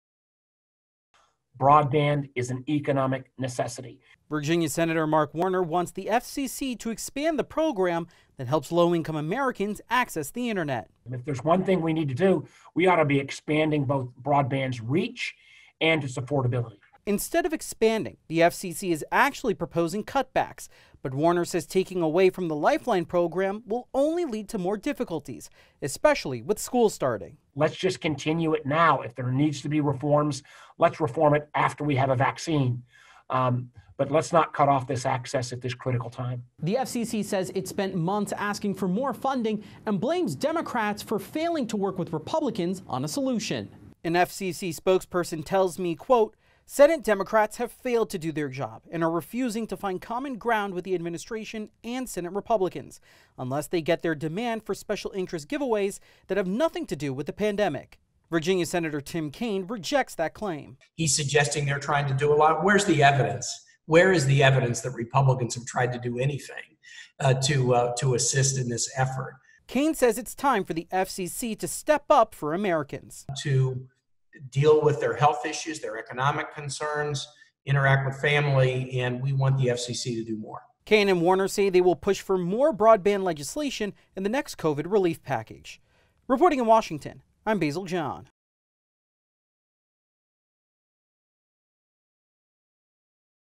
Democrats demand action from the FCC to support Lifeline Program for low-income Americans
Nexstar's Basil John reports.